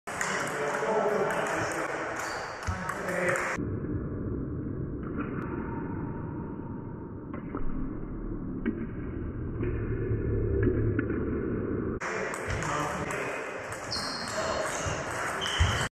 Calling a let on an ace serve 🙈
Action from the Orsted Grimsby, Cleethorpes and District Table Tennis Association.